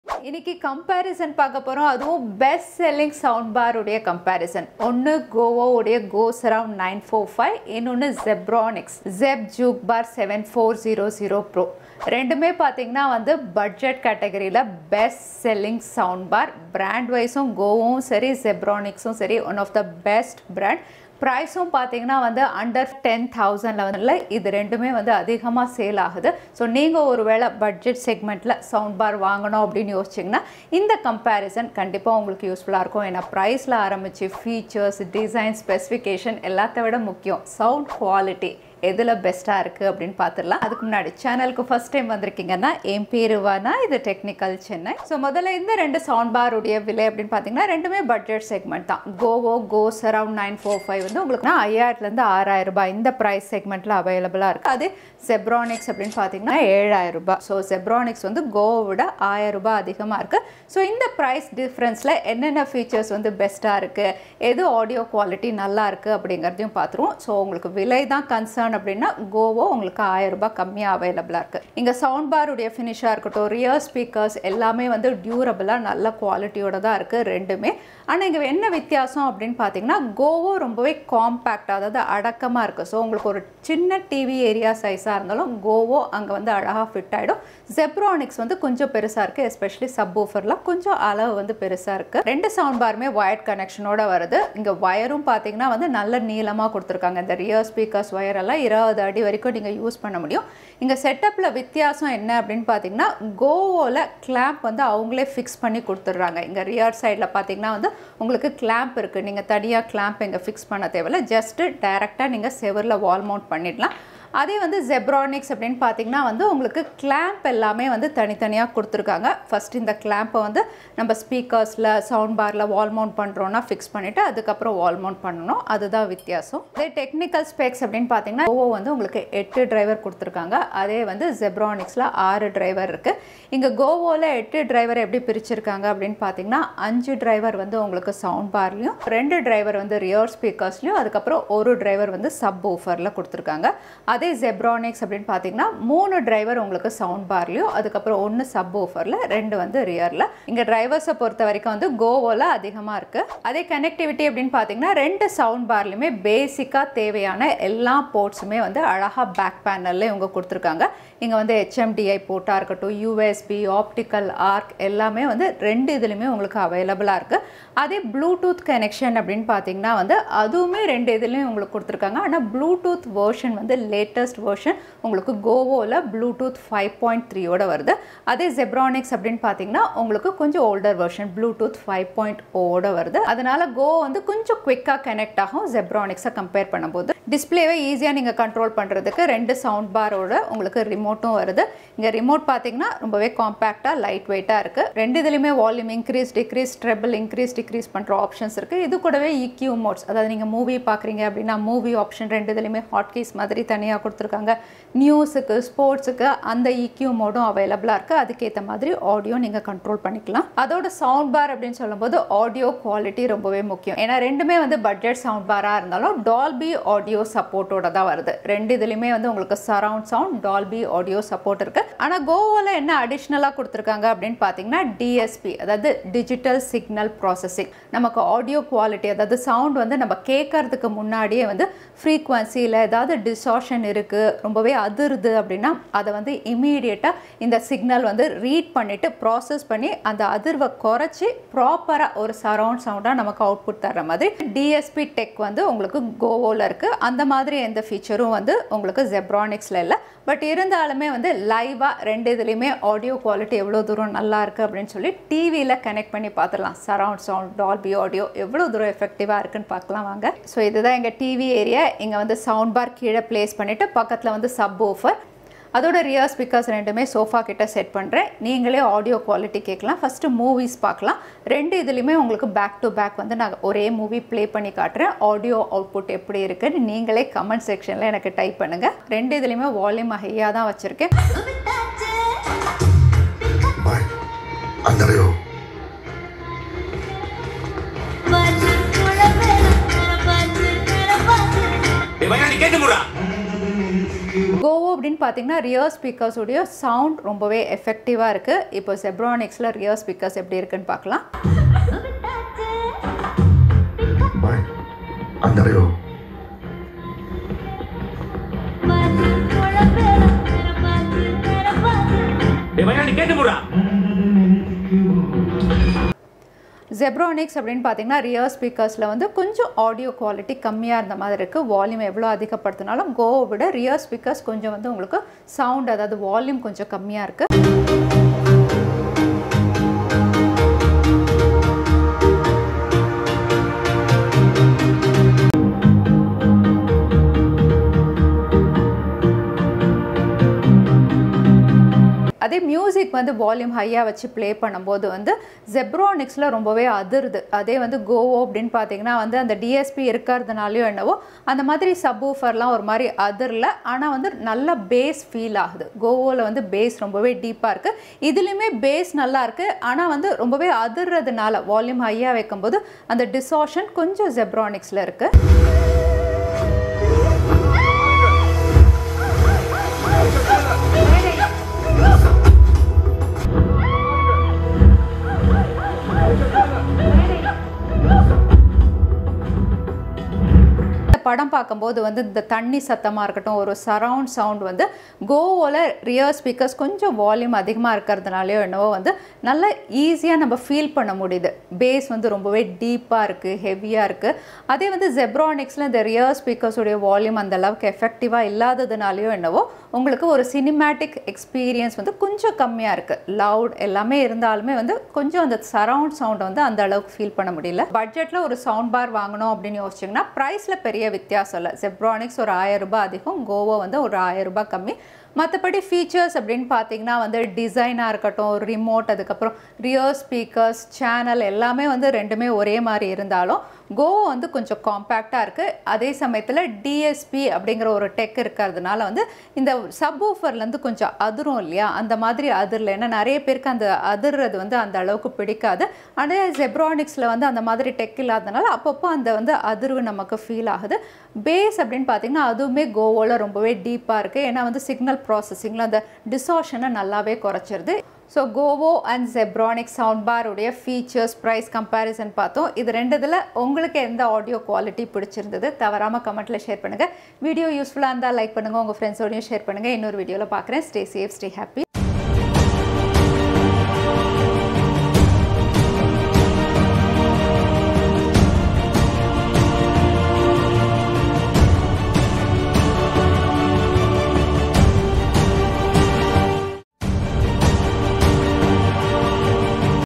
BEST Soundbar UNDER ₹8000 | GOVO GoSurround 945 V/S ZEBRONICS 7400
Welcome to our in-depth review of the GOVO GoSurround 945 Soundbar - the ultimate audio upgrade for your home entertainment! Enjoy your movie nights and gaming sessions with 120 watts of power, 3D surround sound, and a wireless 5.25" subwoofer for deep, rich bass. GoSurround 945 soundbar is perfect for movie enthusiasts, gamers, music lovers.

To Buy

Key Features:

✅ 120W Power for immersive sound
✅ 5.1 Channel 5 x 2" Explosive Drivers
✅ 5.1 Channel with a 5.25" Wireless Subwoofer
✅ Bluetooth v5.3 for seamless connectivity
✅ Multiple Connectivity: AUX, USB
✅ Stylish Remote with 3 Equalizer Modes
✅ LED Display and Wall Mountable

00:00 Introduction
00:40 Features
04:12  GOVO GoSurround 945 V/S ZEBRONICS 7400 Sound Test